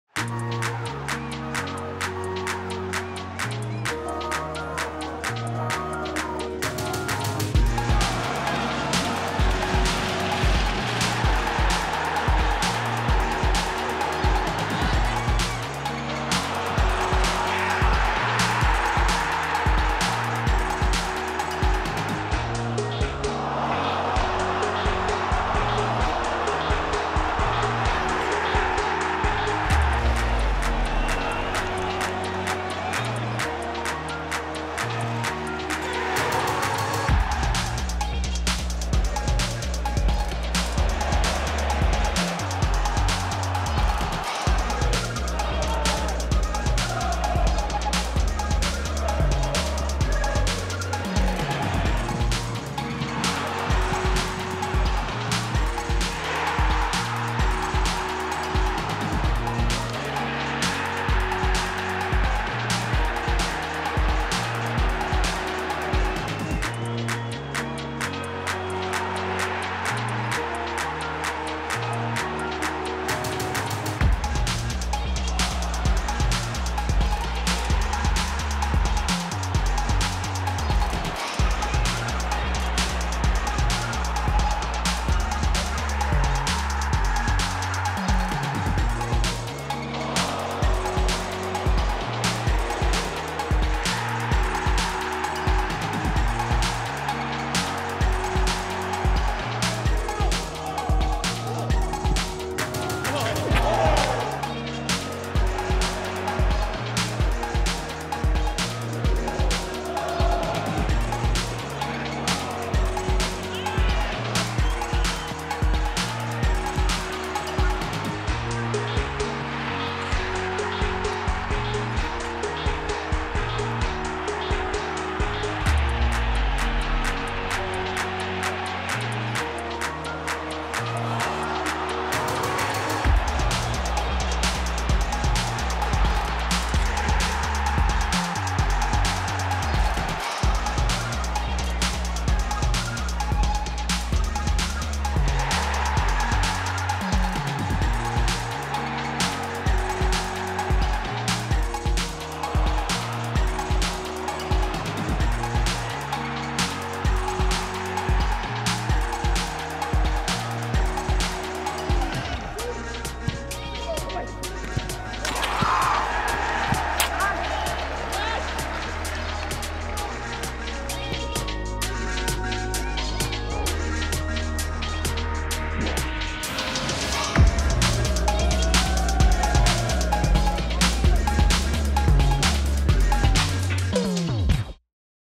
Best of: Roberto Firmino 19/20 | Premier League Champion
Roberto Firmino enjoyed another incredible season spearheading Liverpool's front three and he was pivotal to ending Liverpool's 30 year wait for the league title, enjoy his best bits.